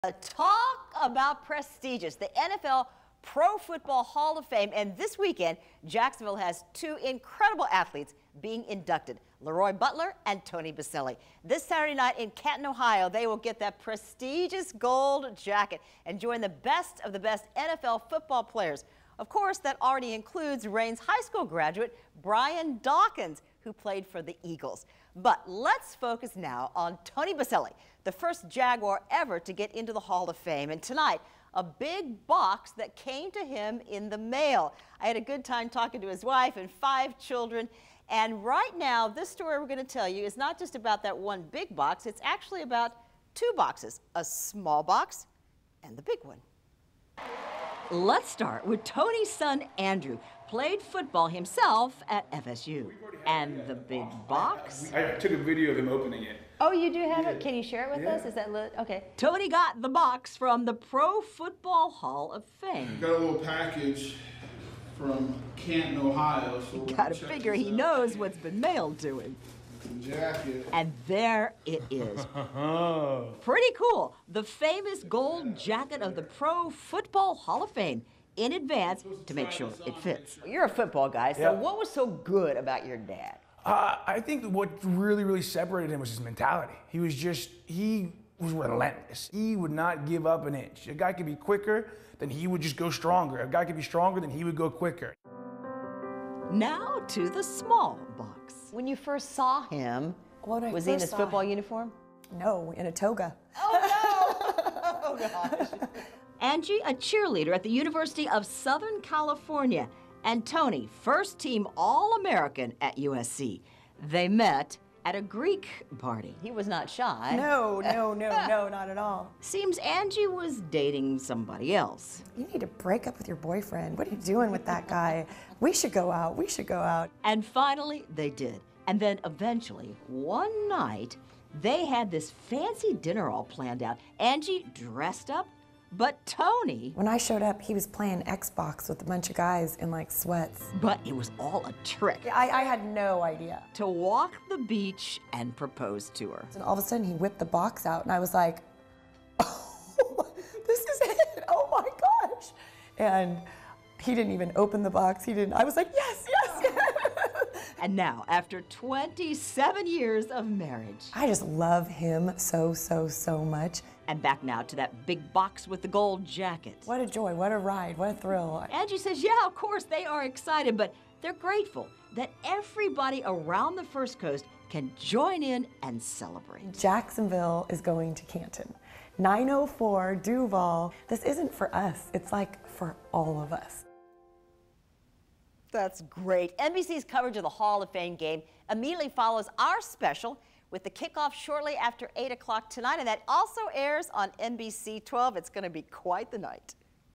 Two boxes with Jaguars Hall of Famer Tony Boselli
One big box from Canton, and a smaller box involving his wife.